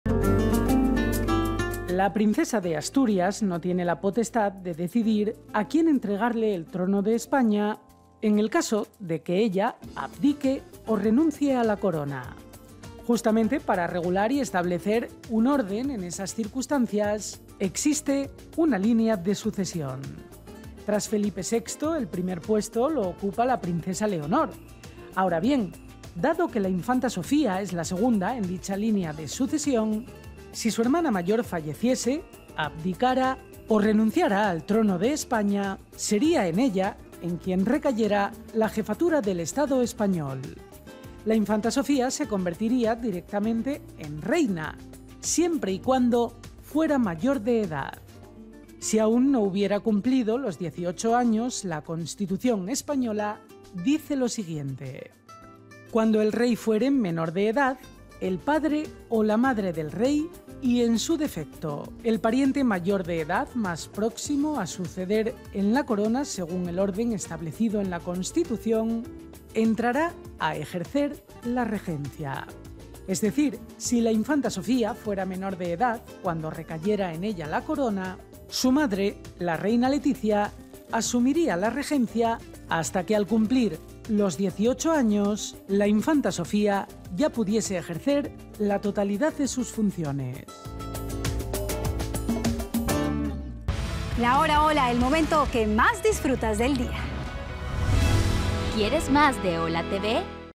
¿Podría la princesa Leonor ceder su papel de reina a su hermana, la infanta Sofía? | ¡HOLA! TV
La princesa de Asturias no tiene la potestad de decidir a quién entregarle la corona en caso de que ella rechace o renuncie su título de reina. Aunque la infanta Leonor es la primera en la línea de sucesión al trono de España y la infanta Sofía es la segunda en el listado, si Leonor renunciara al trono, la infanta Sofía sería proclamada reina, siempre y cuando sea mayor de edad. La reina Letizia sería regente si sus hijas fueran reinas antes de los 18 años.